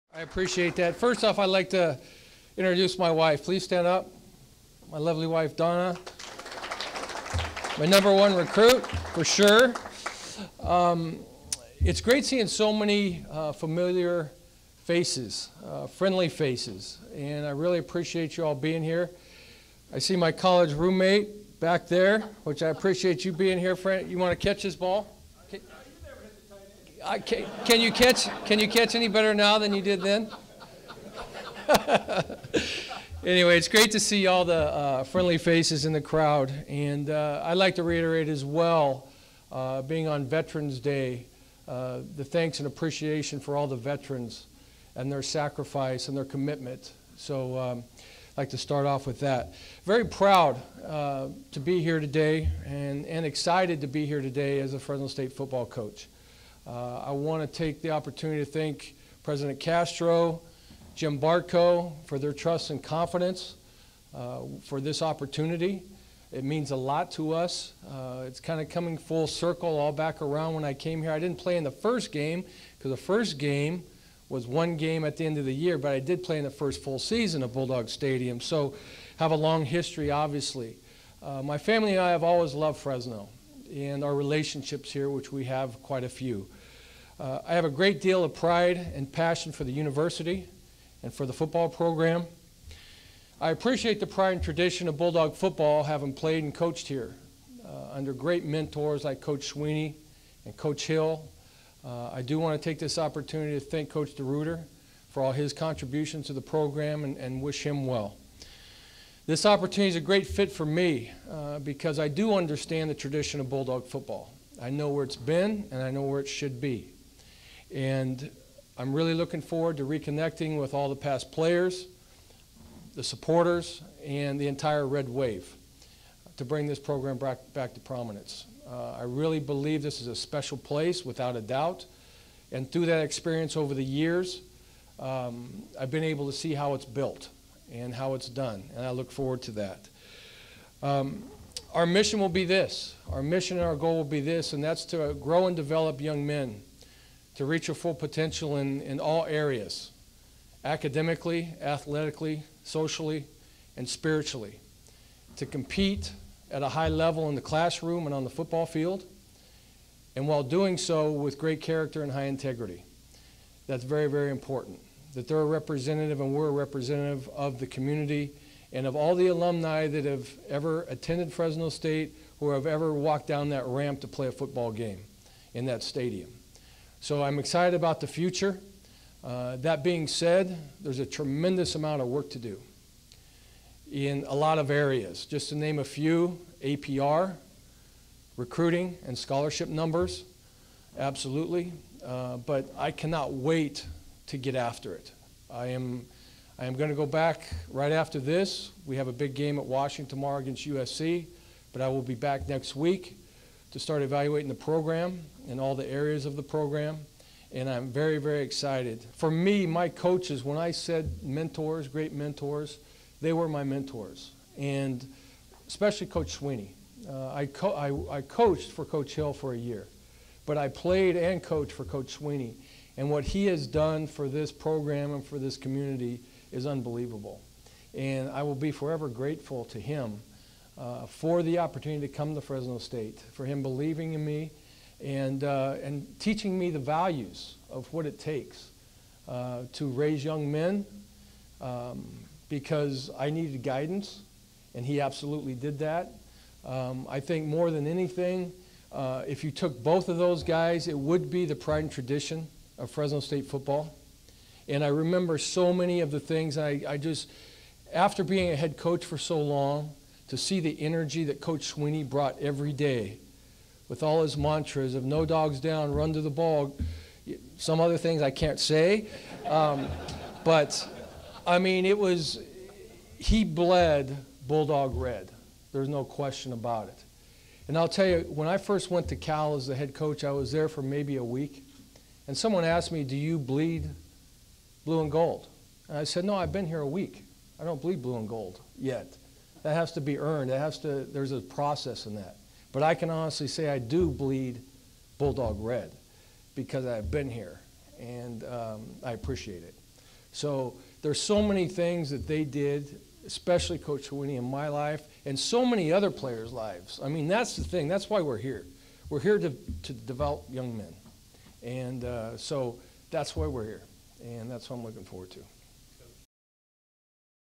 Jeff Tedford Fresno State news conference
Jeff Tedford is introduced as the new head football coach at Fresno State.